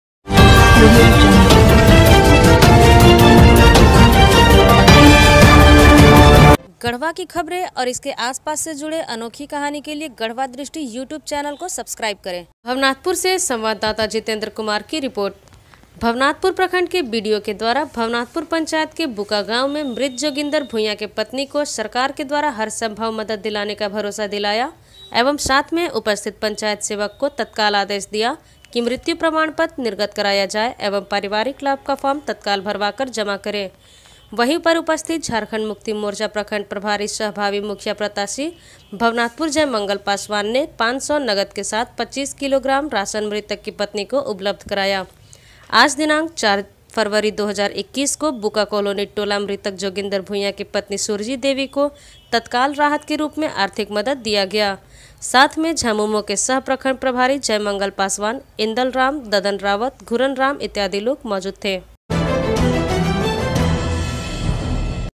भवनाथपुर। गढ़वा। प्रखंड के बूका गांव में मृत परिवार को सरकार के द्वारा हर संभव मदद का भरोसा दिलाया
Beraking News। भवनाथपुर। गढ़वा। भवनाथपुर प्रखंड के बीडीओ के द्वारा भवनाथपुर पंचायत के बुका गांव में मृत जोगिंदर भुइयां के पत्नीको  सरकार के द्वारा हर संभव मदद दिलाने का भरोसा दिलाया। Garhwa News। Garhwa

गढ़वा दृष्टि के अनोखी कहानी देखे